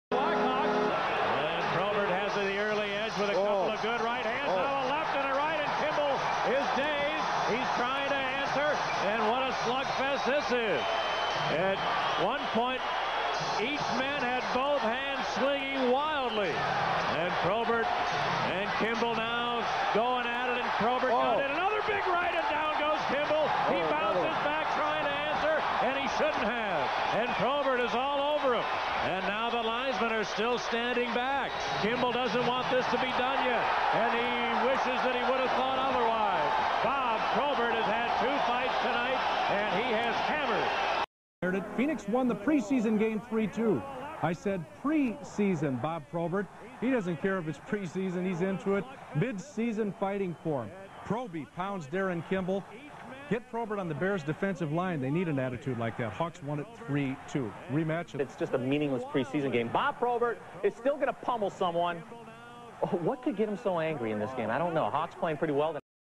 Bob Probert TKO's Darin Kimble Round 2
Bob Probert ( Chicago Blackhawks ) knocks down Darin Kimble ( Phoenix Coyotes ) - NHL hockey fight Sep 19, 1997